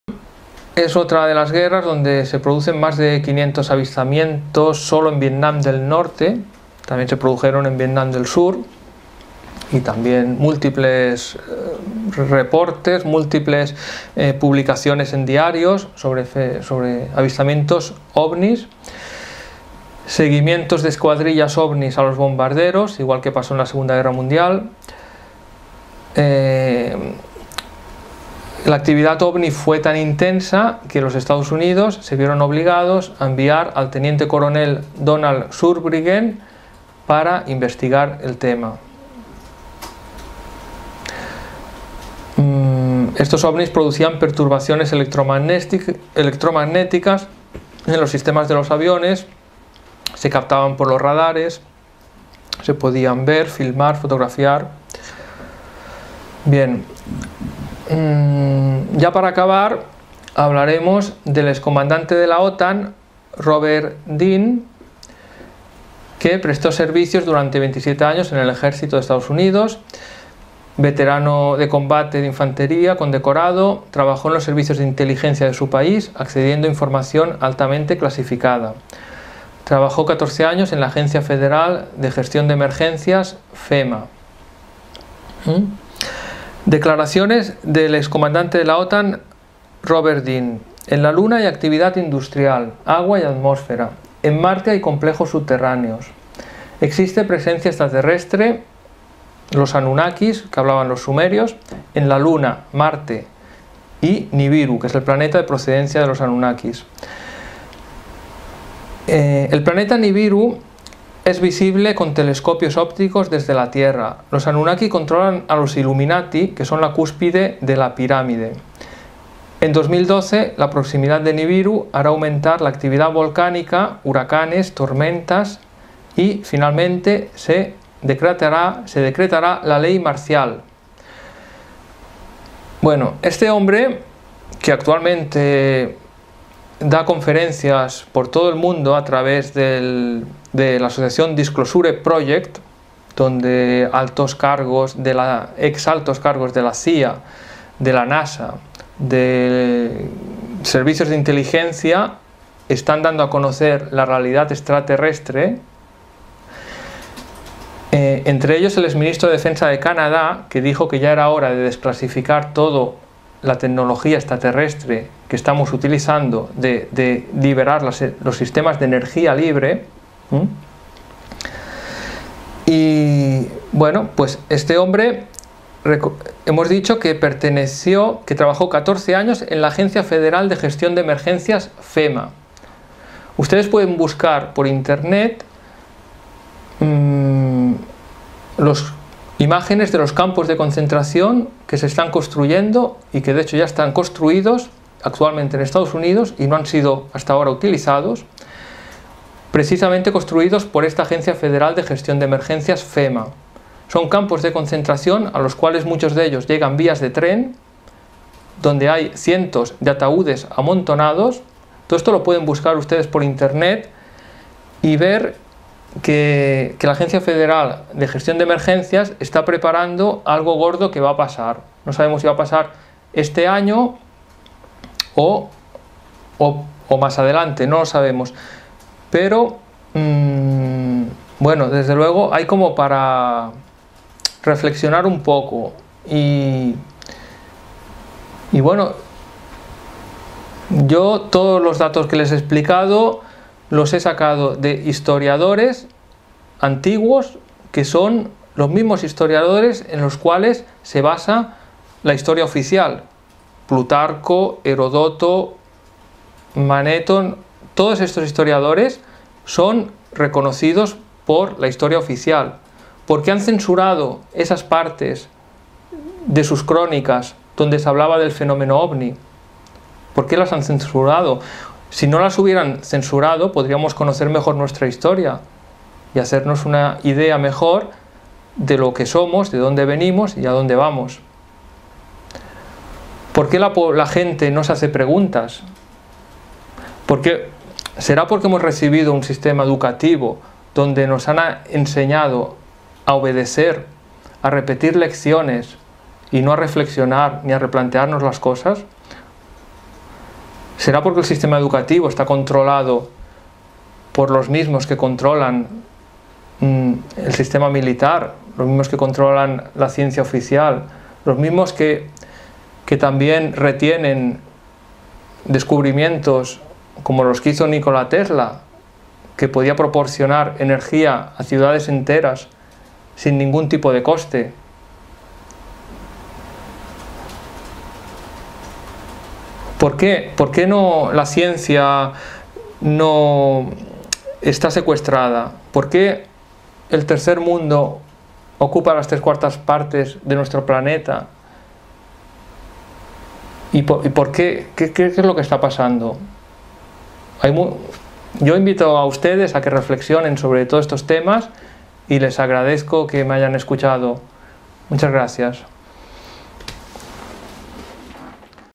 Los Dioses de la Guerra 6/6
Presencia OVNI en los conflictos bélicos. Conferencia de David Ayen 02/01/2012.